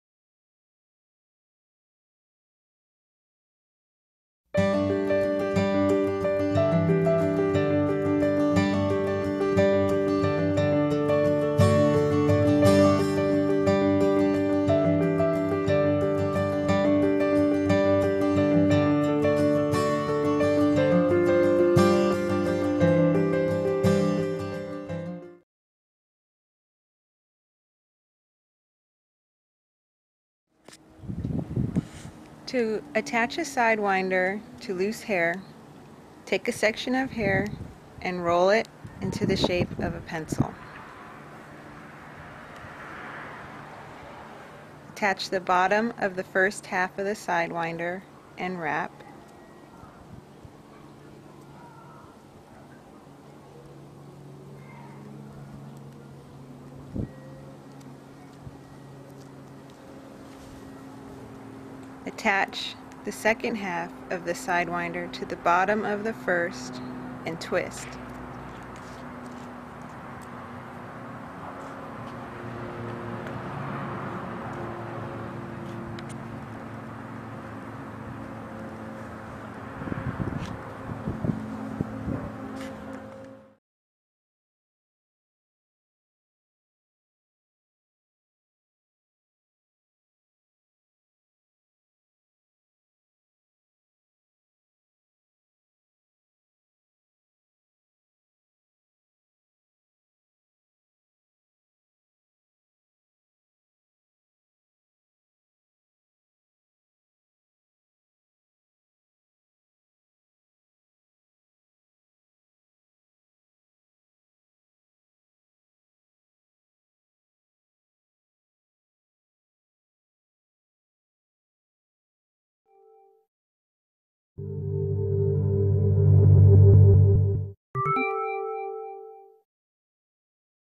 How-To Video for Sidewinder Hairholders
Create cute girls hairstyles with Sidewinders, the hair beads, hair clips & barrettes alternative for every hair length.